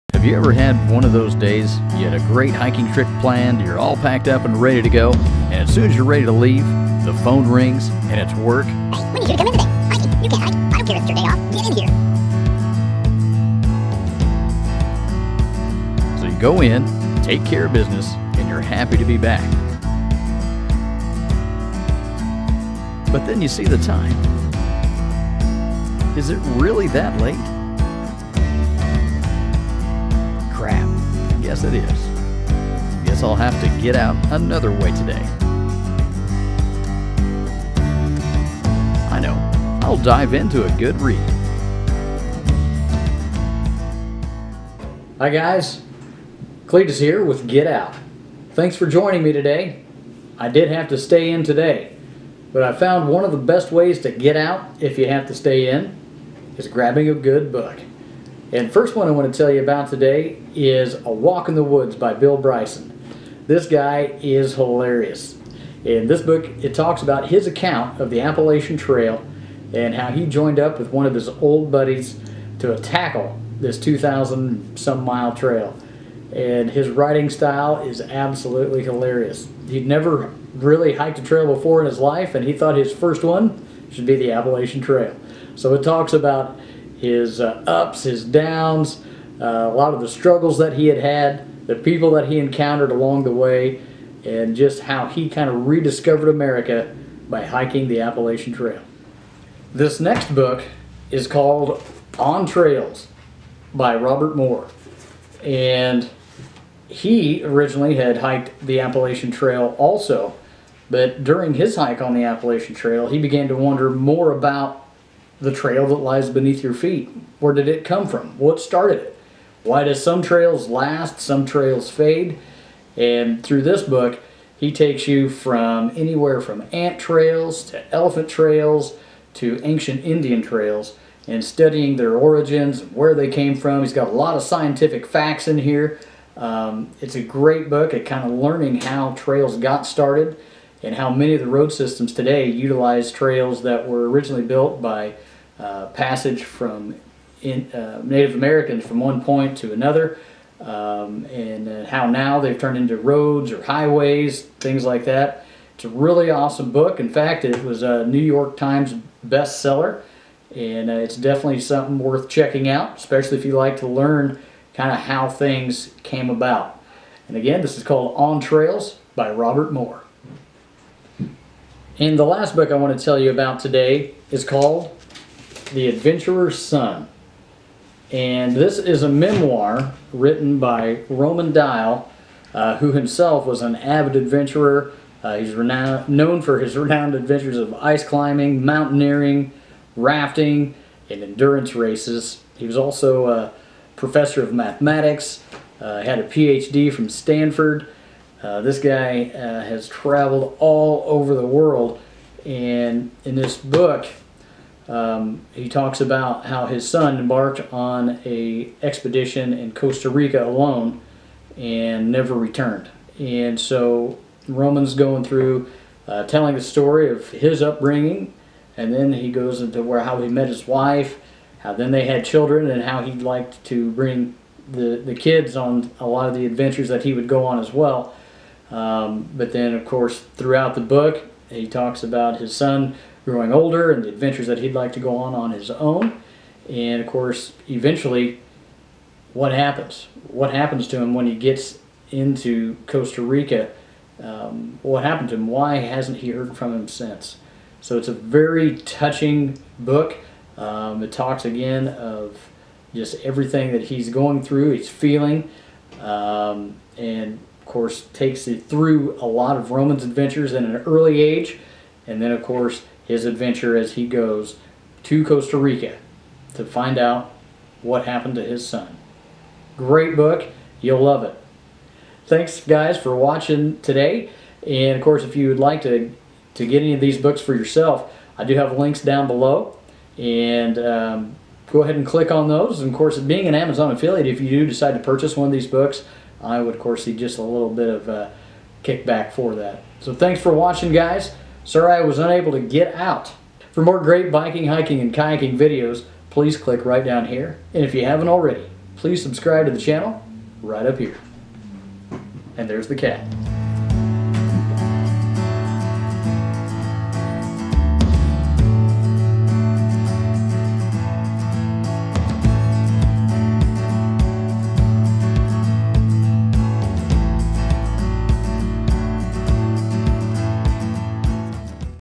How to Get-Out when you have to Stay-In!
In this video I talk about 3 great books to help you escape even if unfortunately you have to stay indoors. Each book is an amazing read, and all are based on real events, below is a link to each book!
I would like to hear about your favorite books as well!

A Walk in the Woods, Bill Bryson

On Trails , Robert Moor

The Adventurer's Son, Roman Dial